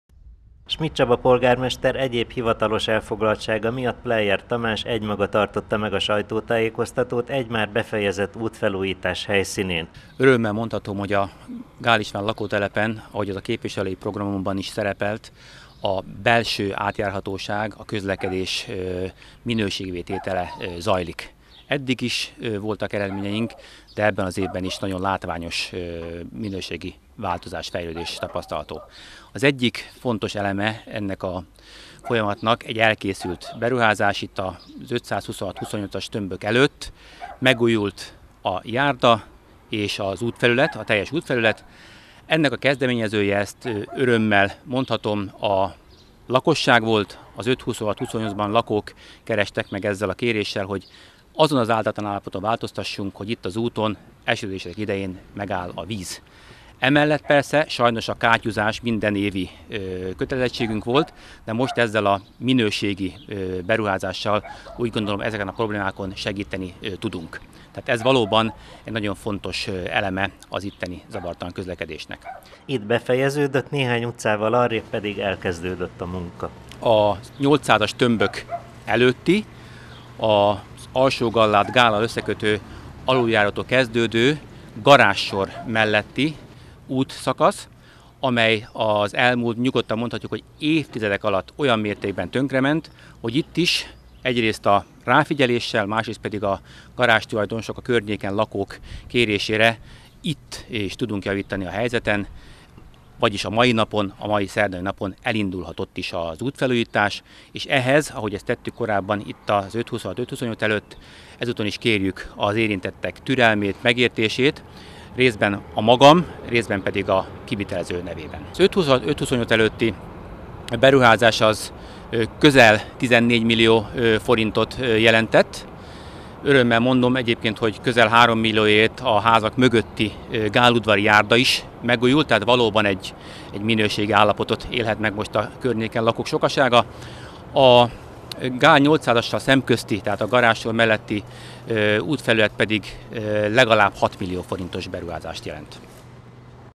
140627 Híradó 06 Útfelújítás kezdődik a napokban a Gál István lakótelepen
Útfelújítás kezdődik a napokban a Gál István lakótelepen. A garázssoron lévő, igen rossz állapotban lévő út kerül felújításra-jelentette be közös sajtótájékoztatóján Schmidt Csaba polgármester és Pleier Tamás a körzet önkormányzati képviselője.